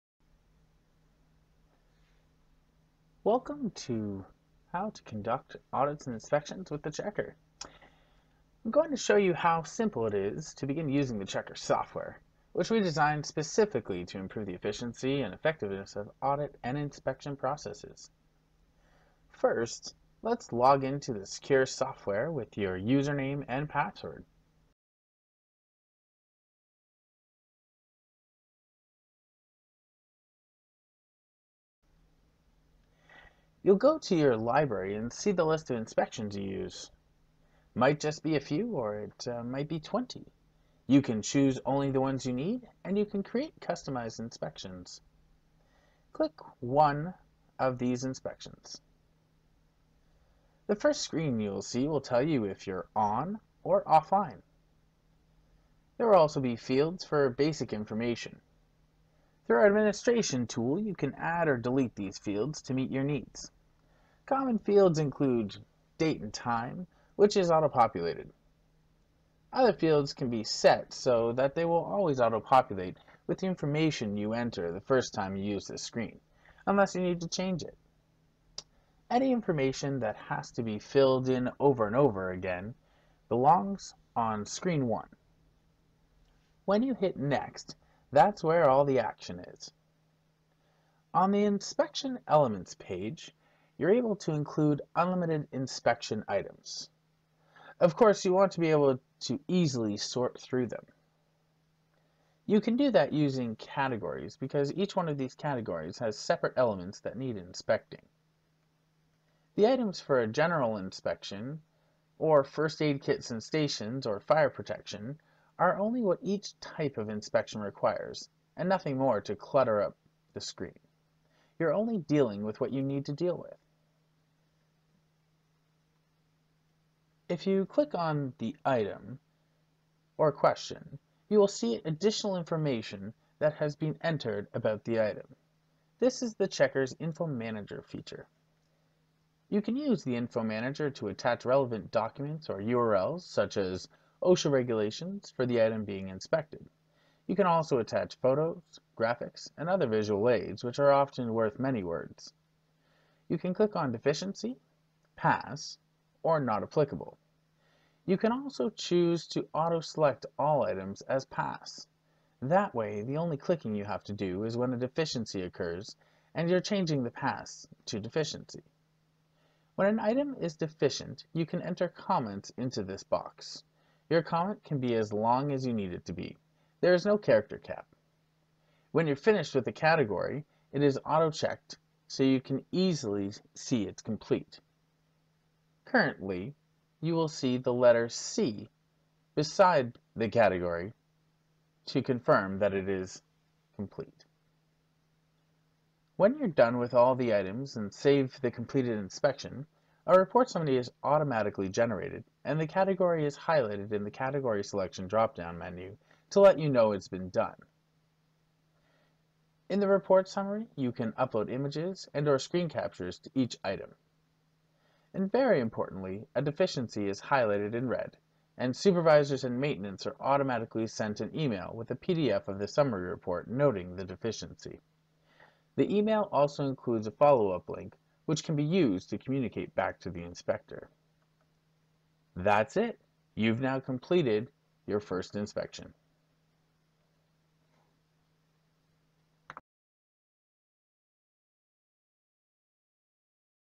How To Complete An Inspection With The Checker Software
With this video, you'll discover how easy and efficient it is to complete an inspection with The Checker software.